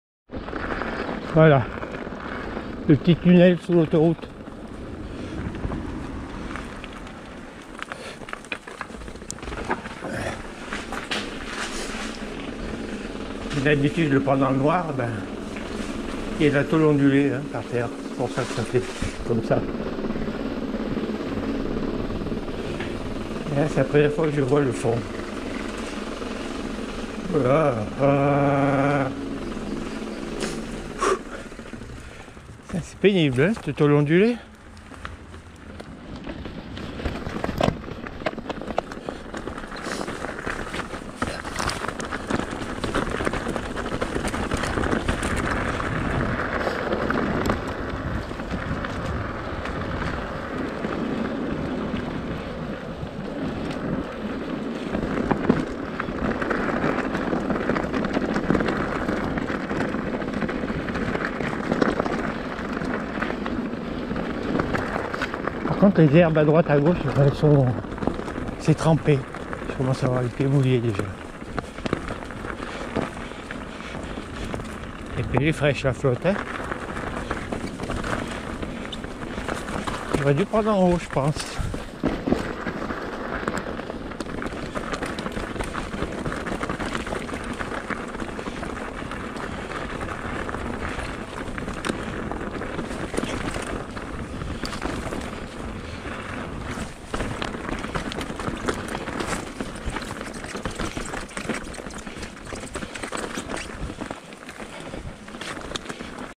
le bon jean 2
de chateauneuf a la plage du rouet via la plaine du romaron et le vallon de valtrede